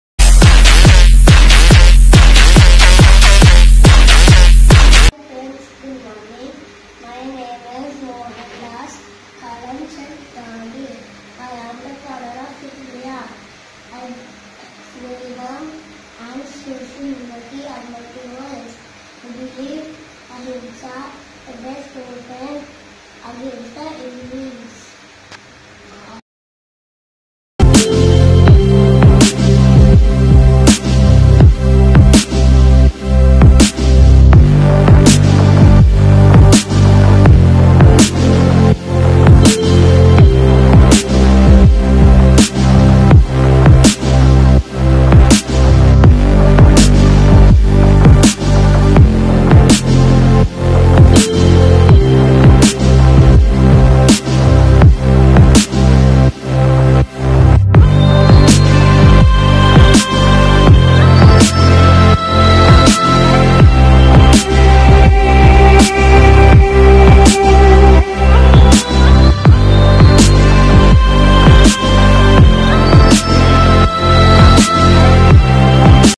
Gandhi Jayanthi Spl 😍 Throw back ...Sweet Memories 🥰🥰
I have dressed up as Gandhi ji in 2017 and now  for a school activity ..Sharing those lovely moments..